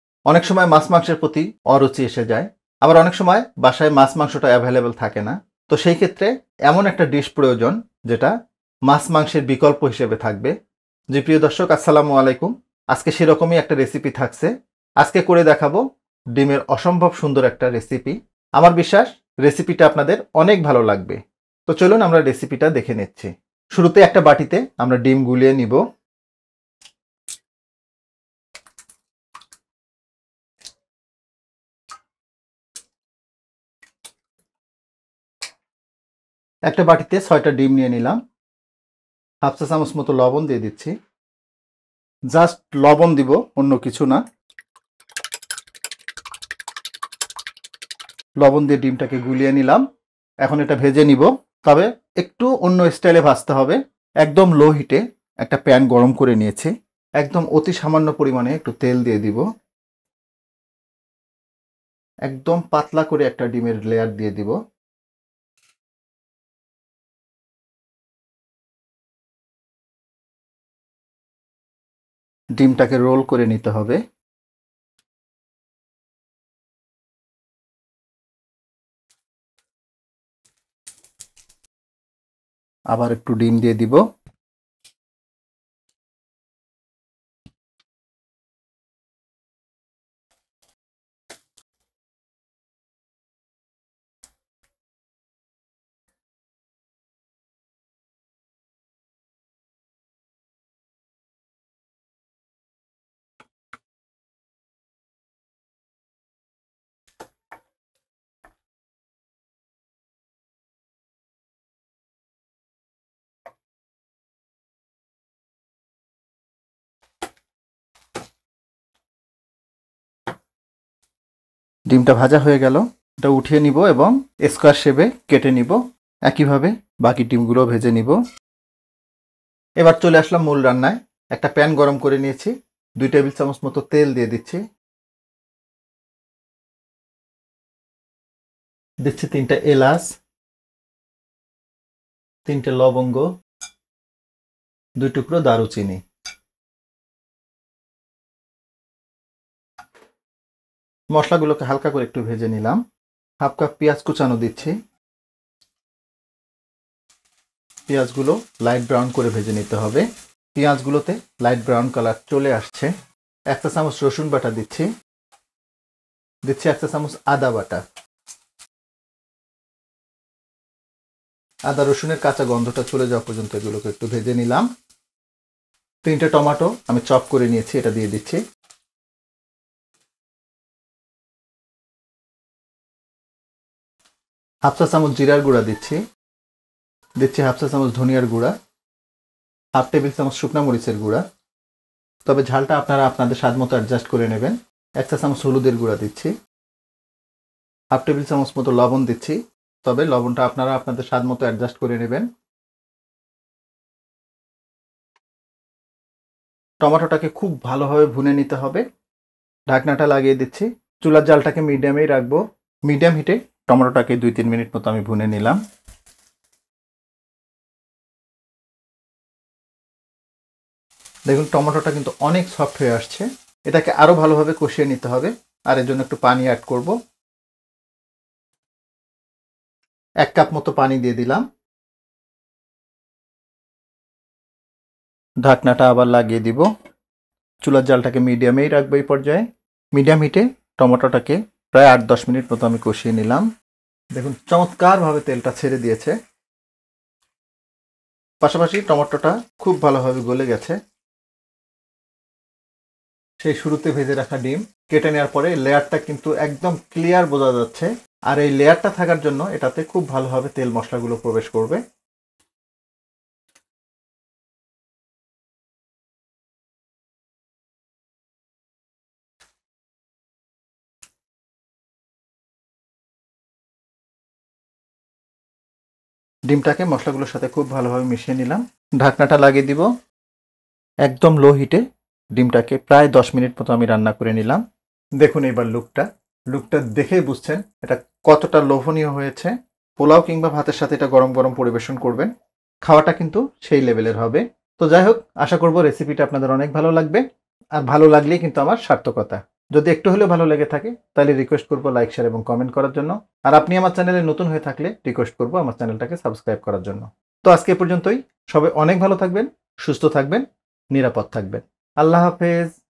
এই নিয়মে ডিম রান্না করলে মাছমাংসের দরকারই হবেনা | New Style Egg Recipe | সম্মান বাঁচানোর রেসিপি
হঠাৎ বাসায় মেহমান আসলো কিন্তু ফ্রিজ ফাঁকা । এইরকম পরিস্থিতিতে ডিমের এমন একটা রেসিপি হতে পারে আপনার মাছ-মাংসের বিকল্প ।
সম্মান বাঁচানোর রেসিপি  🥰 এইনিয়মে ডিম রান্না করলে মাছমাংসের দরকারই হবেনা | New Style Egg Vuna Recipe